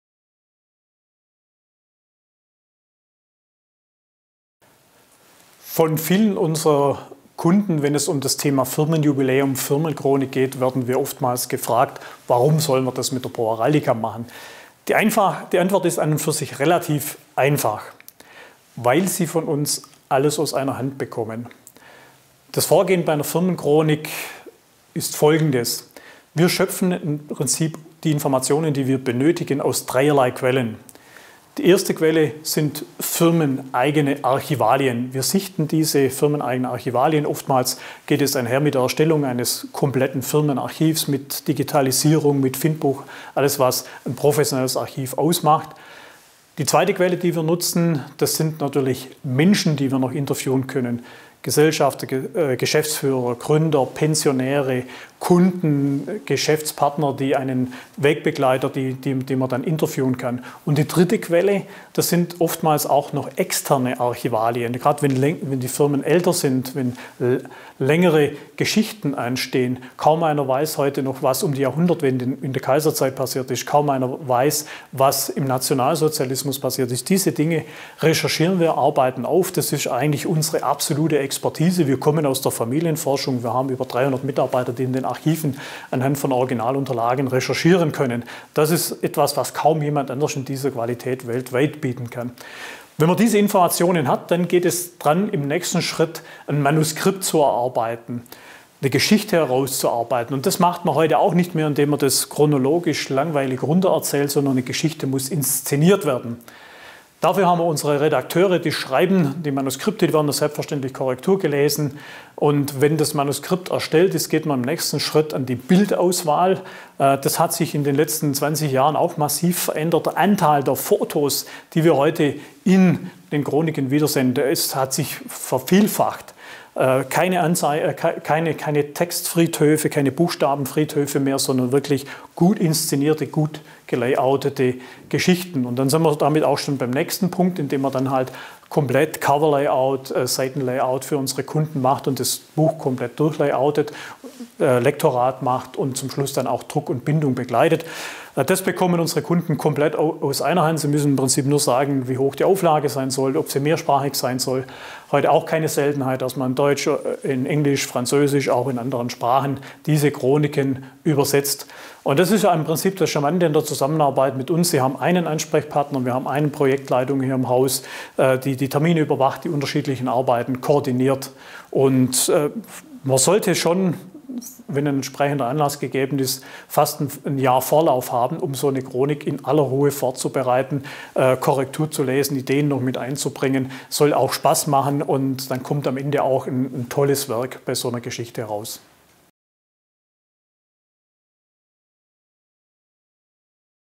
Firmenchronik mit Pro Heraldica - Alles aus einer Hand
Dr. Harald Heimbach erklärt, welche Vorteile es mit sich bringt, eine Firmenchronik im Hause Pro Heraldica erstellen zu lassen. Bei uns bekommen Sie alles aus einer Hand. Im Video werden die wesentlichen Schritte bei der Erstellung erklärt.